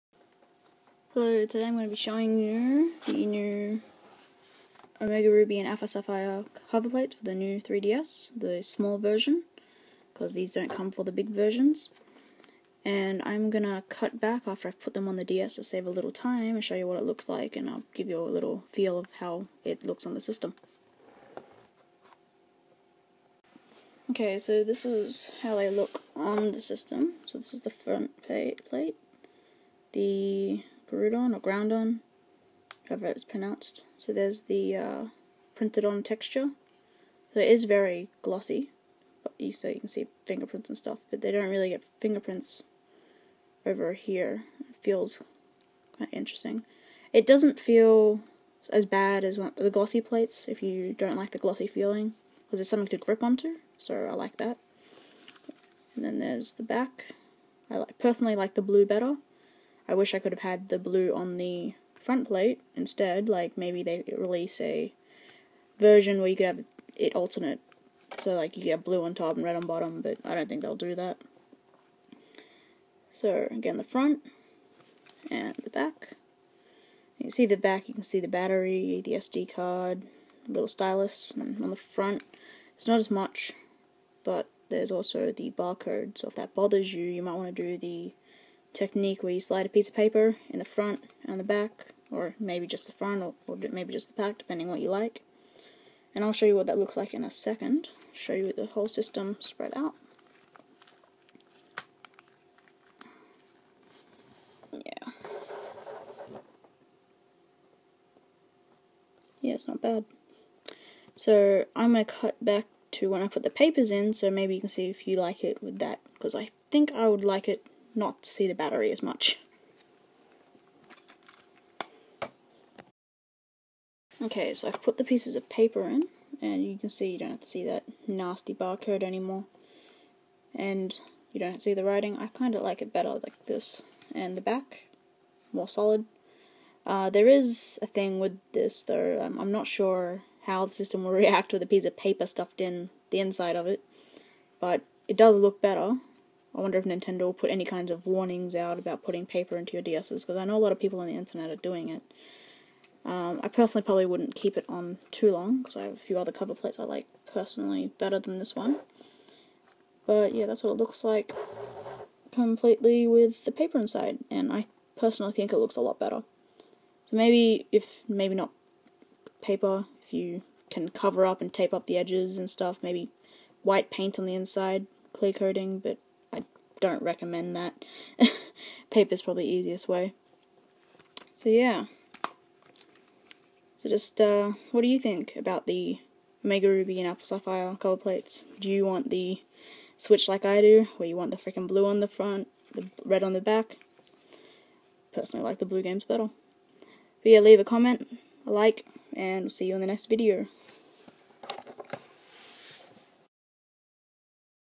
ORAS (Omega Ruby & Alpha Sapphire)Face Plates Review
Review of the ORAS Cover Plates for the new 3Ds. (Available now in Australia)

Next Review: Pikachu Party Cover Plates